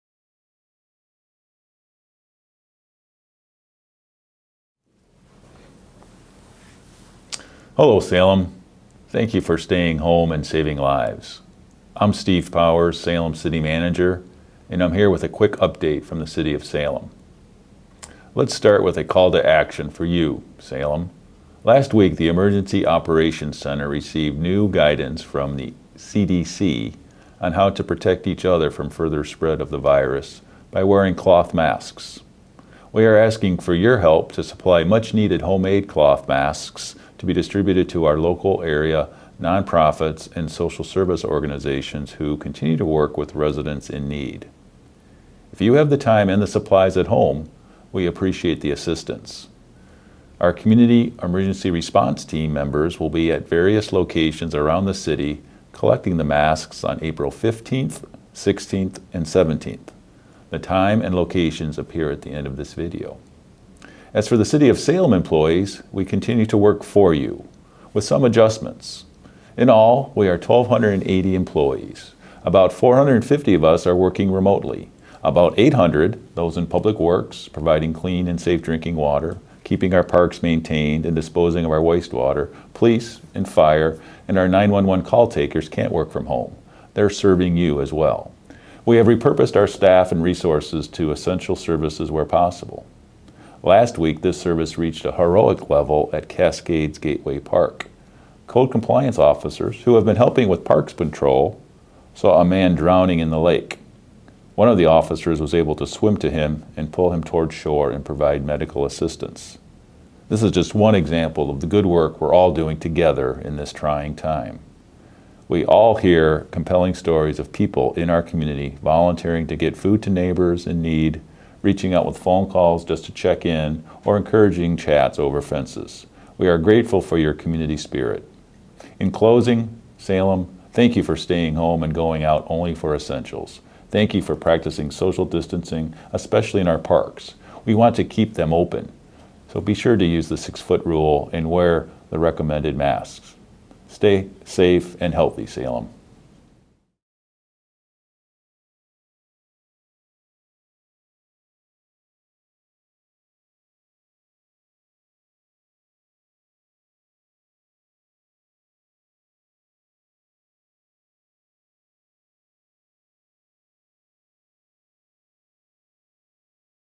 An update from the City of Salem
Salem City Manger provides a services update from the City of Salem for the week of April 13 2020.